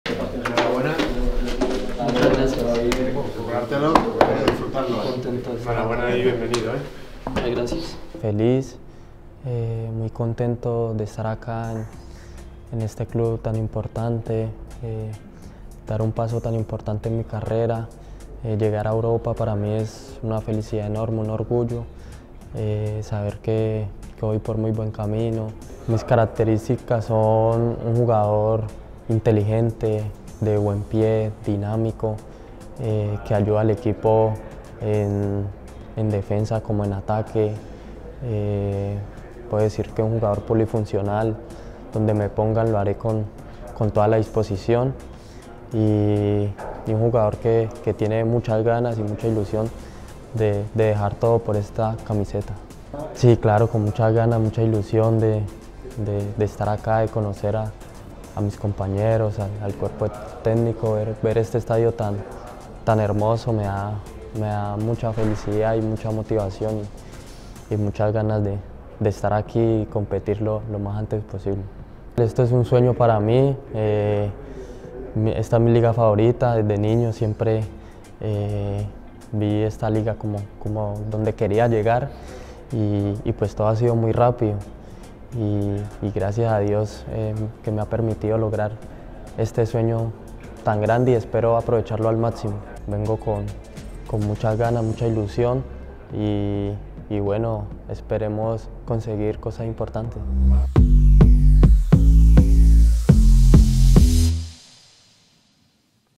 Daniel Luna: "Llegar al RCD Mallorca es un sueño"
El jugador colombiano confiesa su sueño desde niño: jugar en La Liga